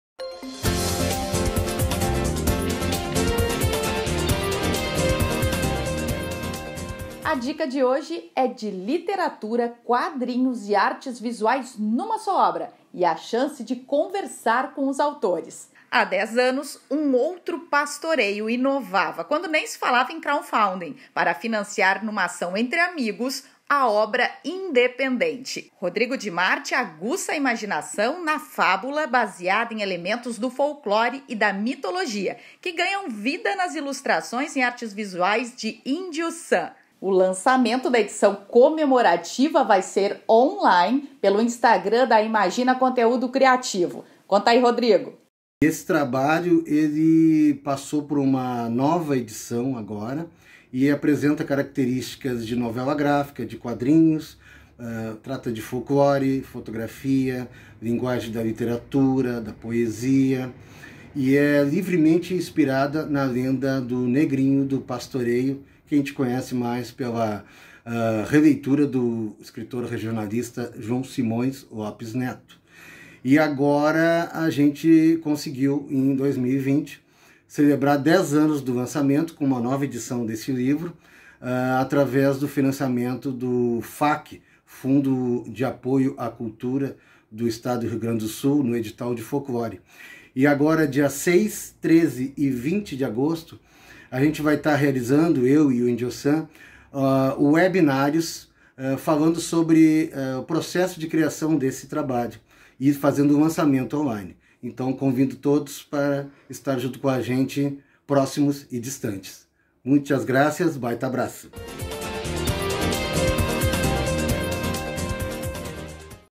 Redação TVE - Lançamento "Um Outro Pastoreio" - 5/8/2020
O lançamento da segunda edição da Graphic Novel “Um Outro Pastoreio”, comemorativa dos dez anos, vai ser online, com conversas com os autores. O escritor e jornalista Rodrigo dMart e o ilustrador Indio San realizam um ciclo de debates, nos dias 06, 13 e 20 de agosto, quintas-feiras, às 19h, no canal do Instagram da Imagina Conteúdo Criativo. (@imaginaconteudo).